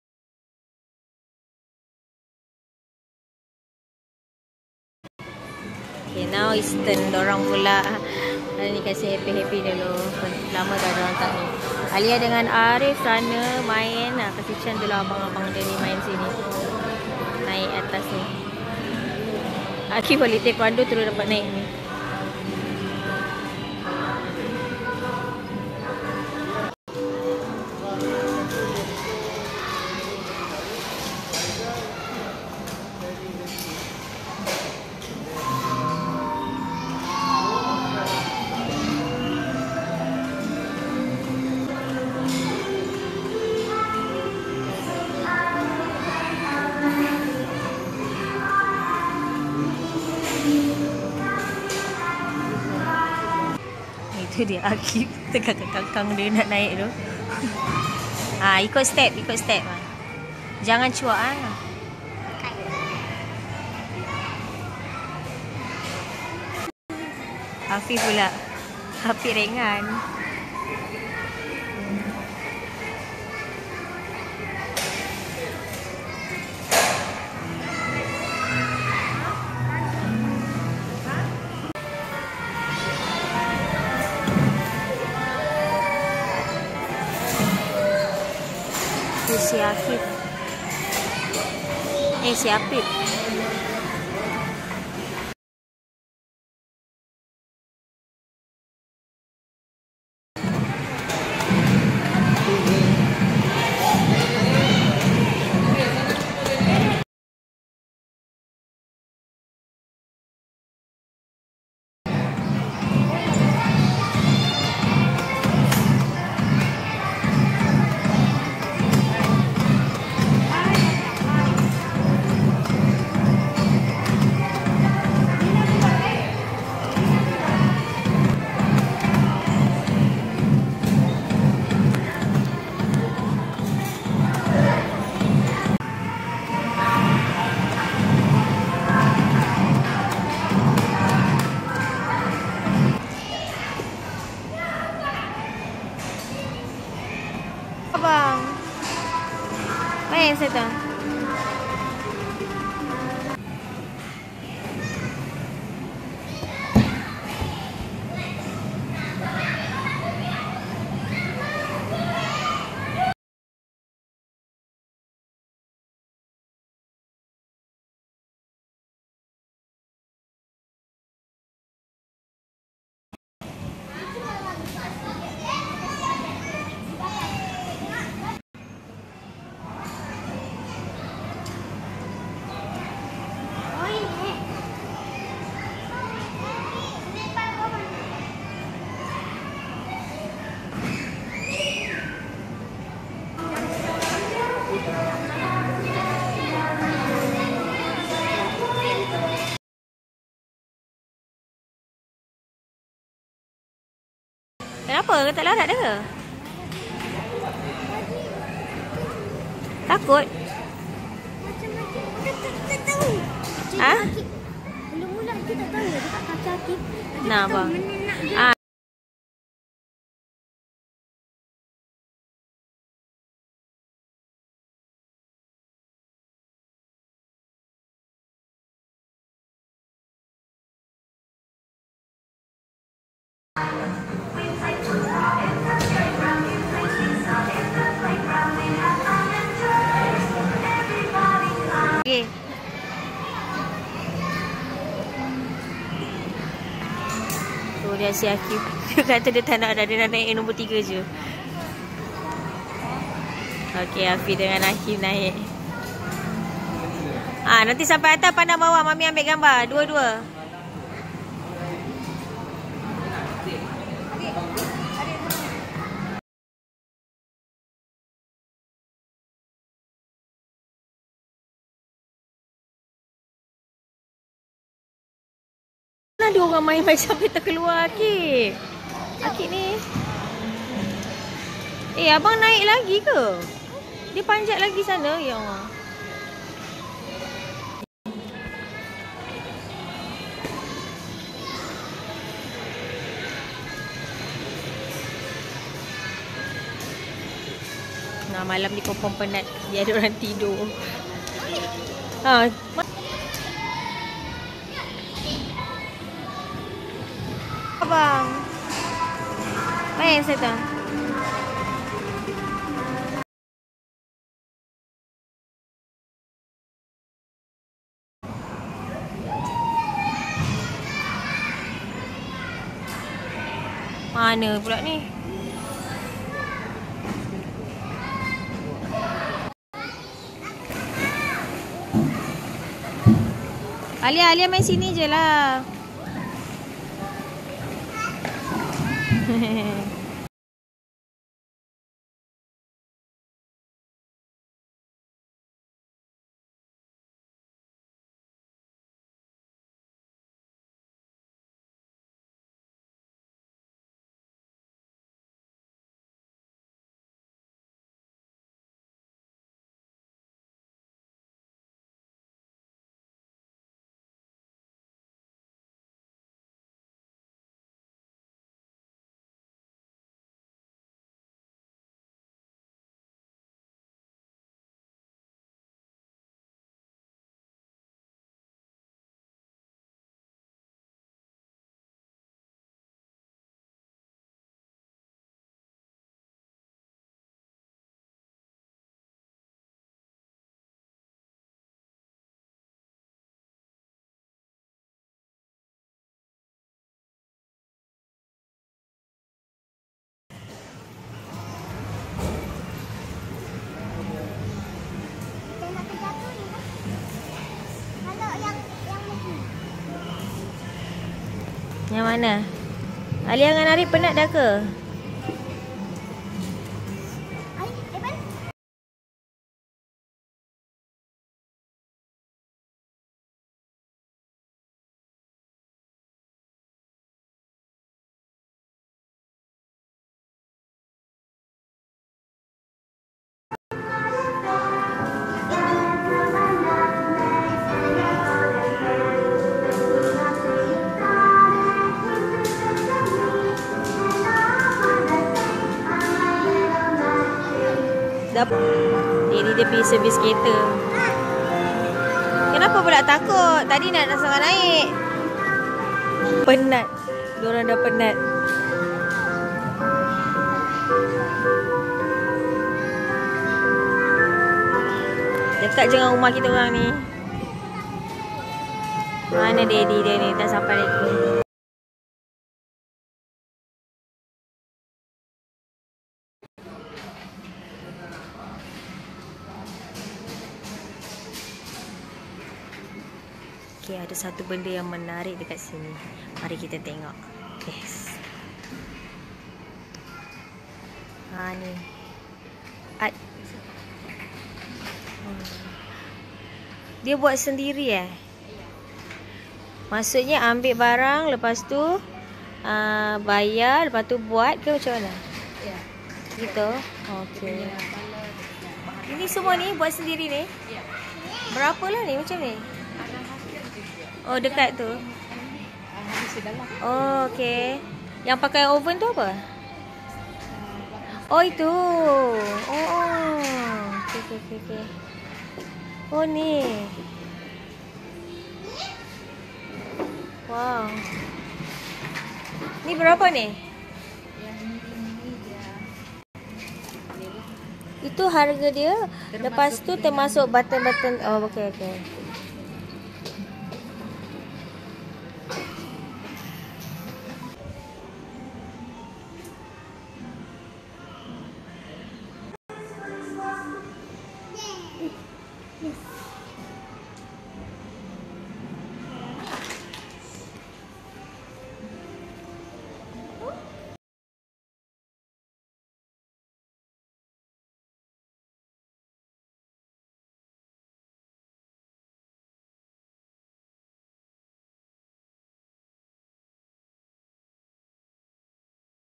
KiDz One Place Mall
Puas main... tiada had masa.. dekat je dengan rumah. Yg perlu ada ke cing 💸 ke cing 💸 ke cing 💸
Asal budak4 HAPPY